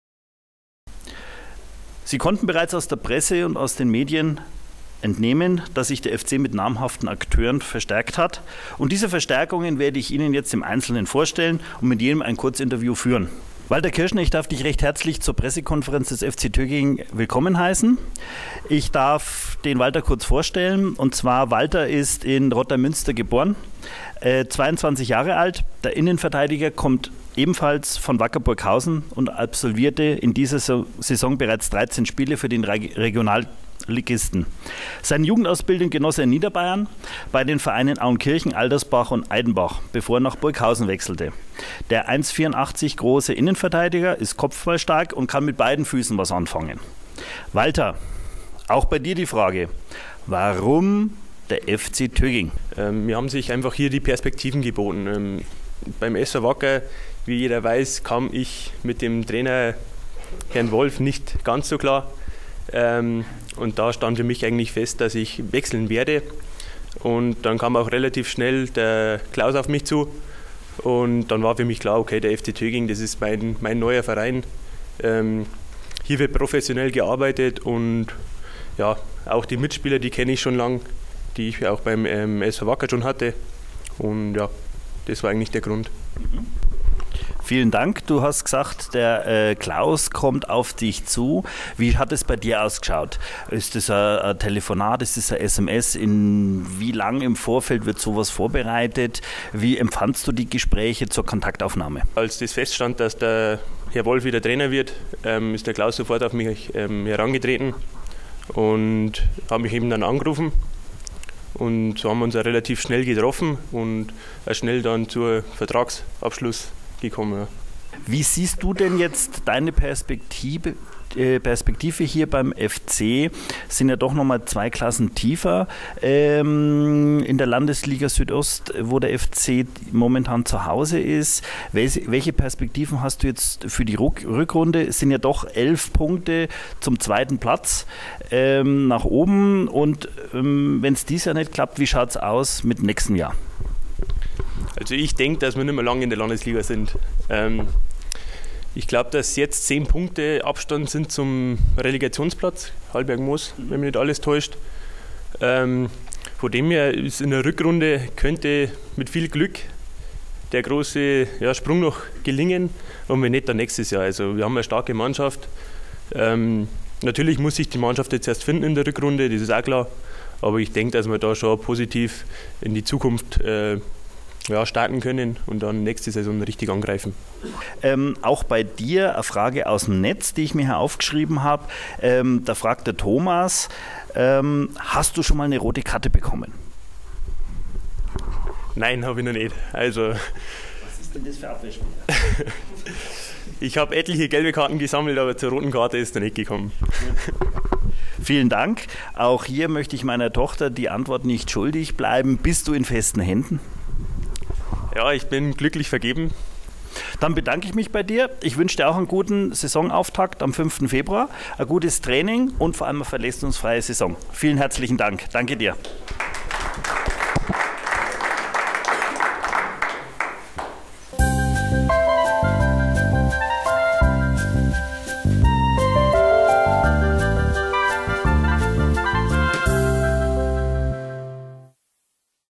Landesligist FC Töging stellt Neuzugang Walter Kirschner vor
Fußball Landesligist FC Töging stellt Neuzugang Walter Kirschner vor, der vom SV Wacker Burghausen nach Töging wechselt.